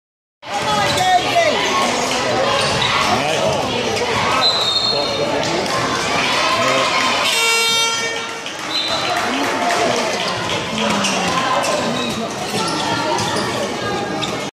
Guernsey Mens Volleyball team in action Island Games 2015
Guernsey Men's Volleyball team in action Island Games 2015